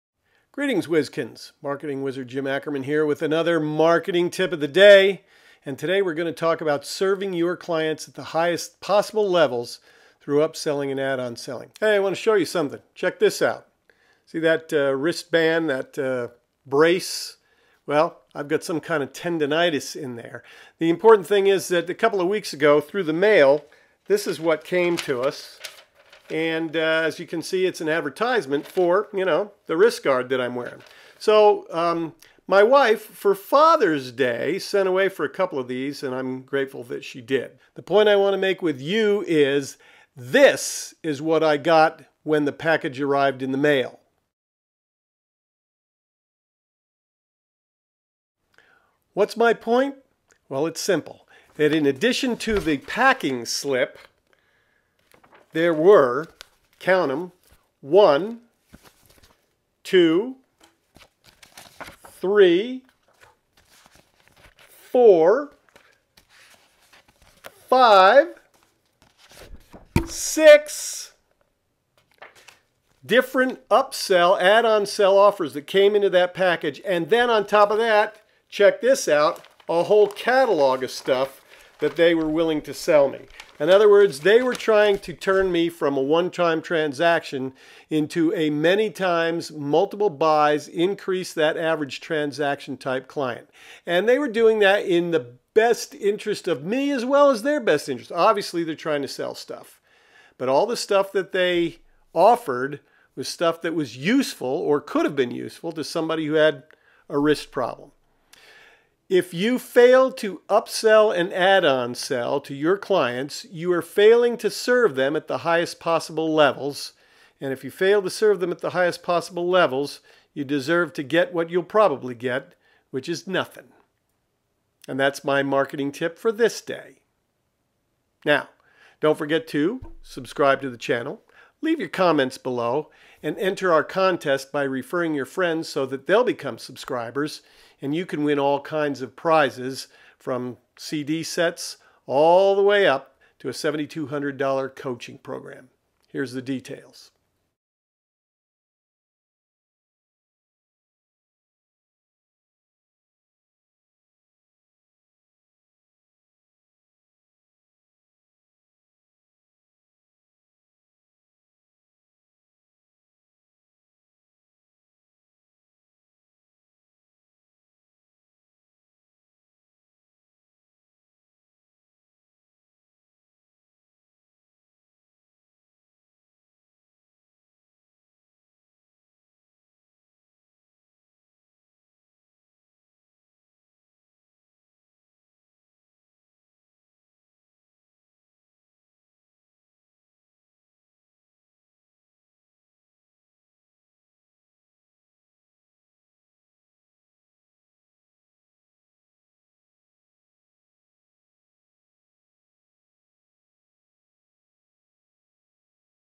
Marketing Tip o'the Day... Add-on Sell
This week, win my X-TREME RETAIL, 3CD set worth $77.00, by referring subscribers to both channels.

And, by all means, take a lesson from the wrist brace folks Marketing Speaker Jim Ackerman talks about in this 30-Second Marketer's Tip o'the Day. It's about getting FREE MONEY, and getting it INSTANTLY!